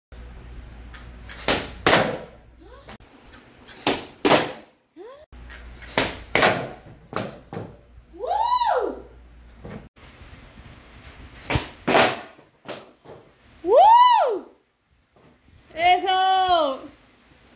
Laserflip en casa
Para poder ganar confianza y hacer el laserflip en obstaculos debo dominarlo en casa.